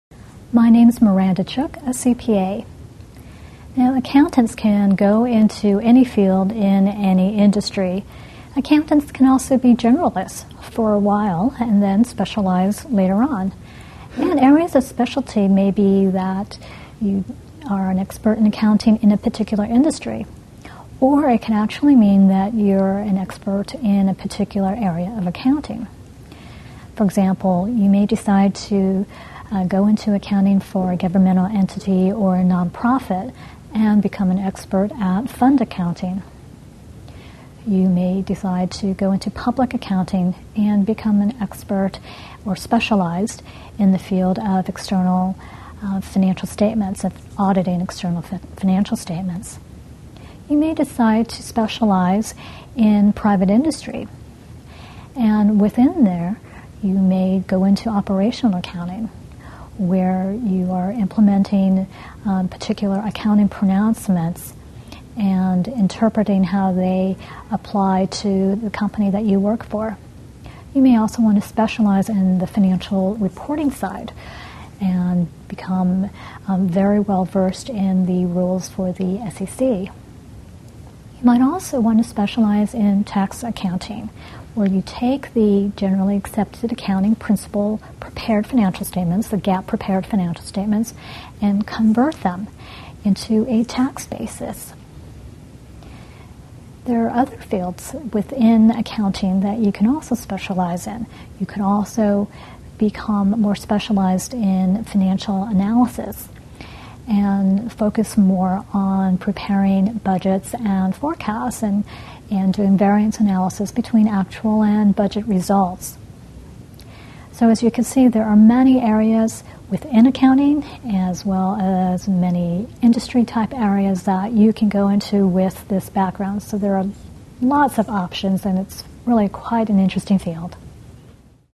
Specialized Fields of Accounting
Specialized Fields of Accounting. Part of the series: Finance & Accounting Facts. Specialized fields of accounting include fund accounting, working with external financial statements, operational accounting, tax accounting or budget preparation. Decide whether to work in the public, private, commercial or governmental fields of accounting with help from a certified public accountant in this free video on accounting careers.